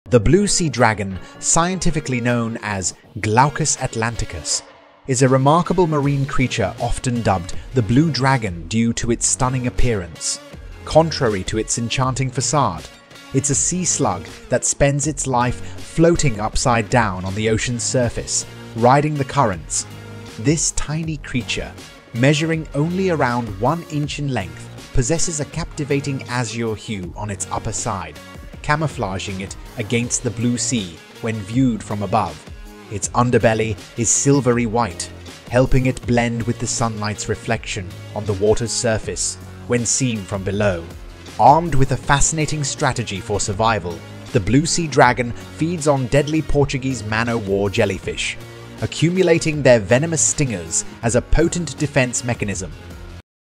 Blue Sea Dragon | The Beautiful But Deadly Blue Sea Dragon
In this video, we'll dive deep into the mesmerizing world of the "Blue Sea Dragon" - scientifically known as Glaucus Atlanticus. This extraordinary creature, often referred to as the "Blue Dragon" or "Blue Sea Slug," may look small and delicate, but don't be fooled by its appearance! Measuring only about 3 cm in length, this tiny marine gastropod packs a powerful punch